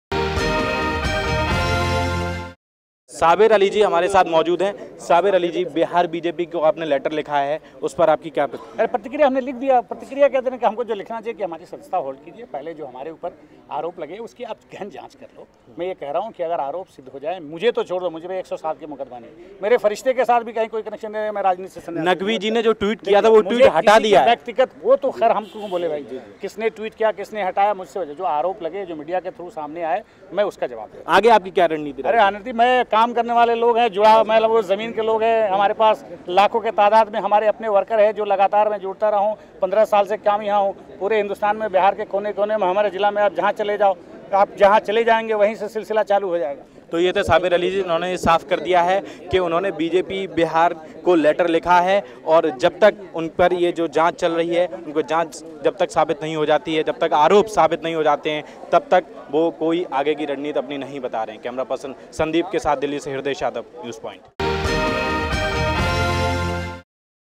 Newspoint Exclusive: In conversation with Former JD (U) spokesperson Sabir Ali
Former Janata Dal (United) spokesperson Sabir Ali, who was expelled by the party for praising BJP's prime ministerial nominee Narendra Modi, has joined the BJP at the party headquarters. He said he joined the party to fight against corruption, inflation and appeasement.
Following this, our correspondent held talks with Sabir Ali ...